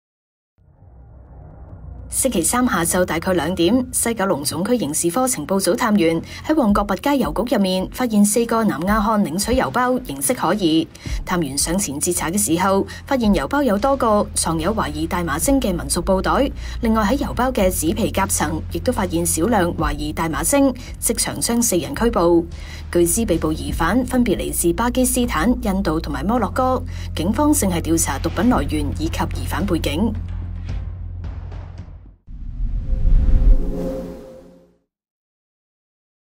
民族布袋藏大麻精 4南亞漢赴郵局收件斷正｜星島頭條｜西九龍總區｜郵局｜郵包｜民族布袋｜大麻｜大麻精｜毒品｜南亞漢｜港聞
今日（4日）下午2時許，西九龍總區刑事科情報組人員發現4名南亞漢於旺角弼街37號的郵局內領取郵包，形迹可疑，之後待他們離開時上前截查。探員打開郵包檢查，發現內有多個民族布袋，裡面藏有思疑大麻精，另外郵包的紙皮夾層亦藏有少量思疑大麻精，於是將4人拘捕，正調查毒品來源及疑犯的背景。